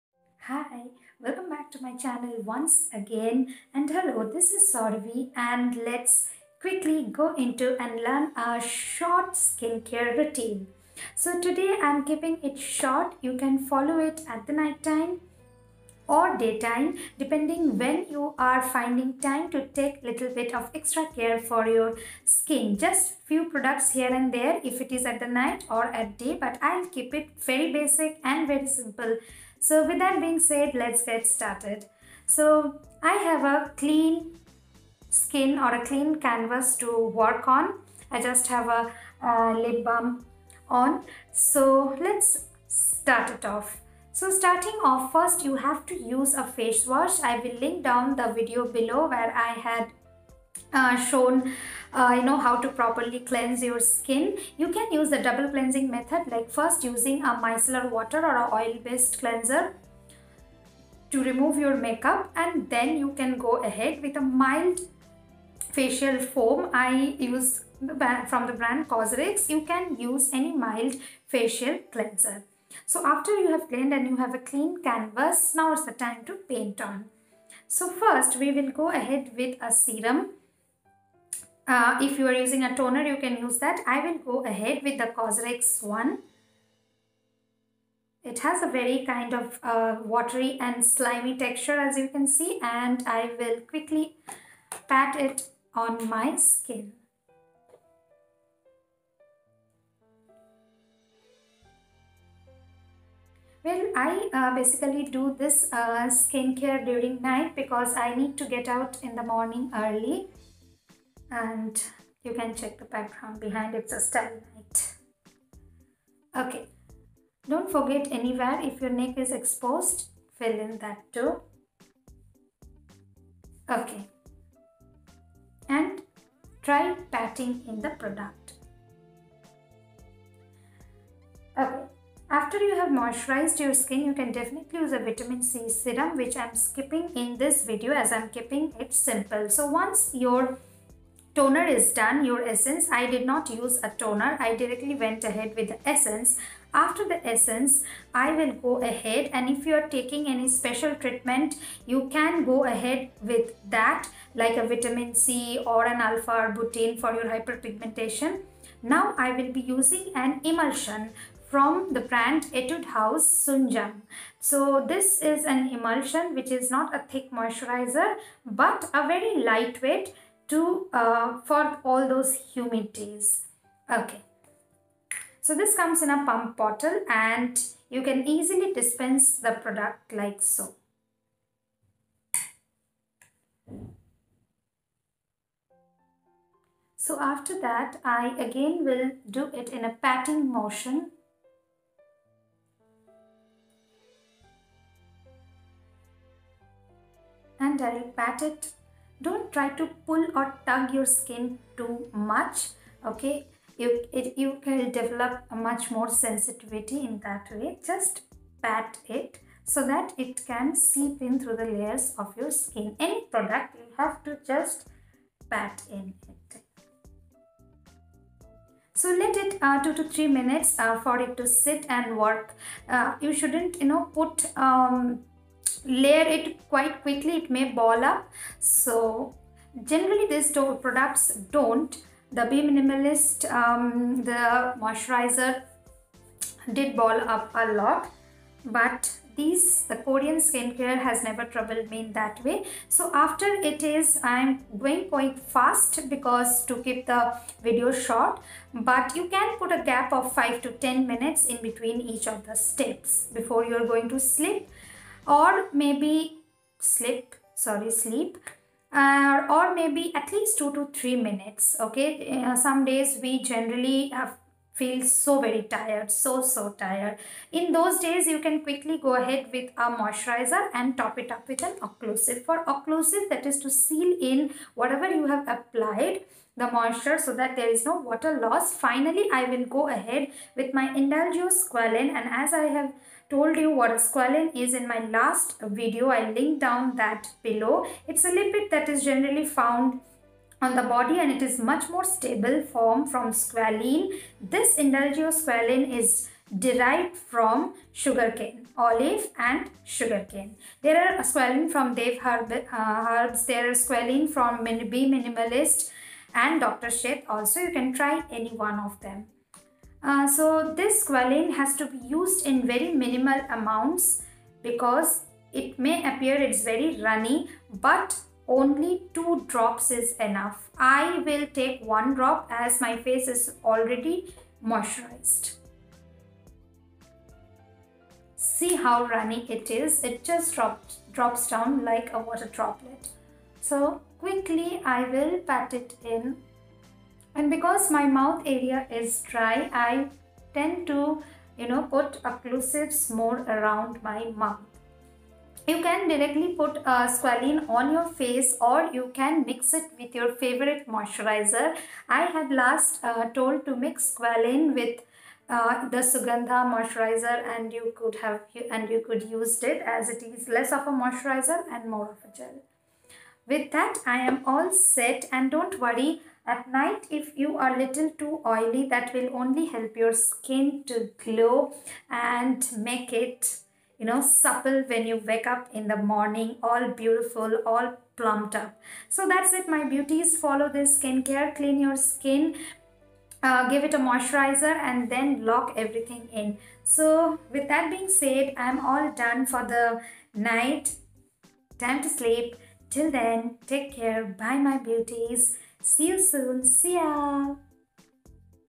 COSRX Snail Mucin , ETUDE HOUSE , INDULGEO - simple Korean skincare routine
or, Beautybarn.in

3. INDULGEO SQUALANE (products links on my old video, get to know what is SQUALANE!)

also, DEVE HERBES SQUALANE
5ML, 10ML, 15ML, 30 ML available)

What Is Squalane? Squalane is a derivative of squalene and is much lighter (and thus a better option for acne-prone skin).

Welcome back, n if you are new thanks again for stopping by .. :)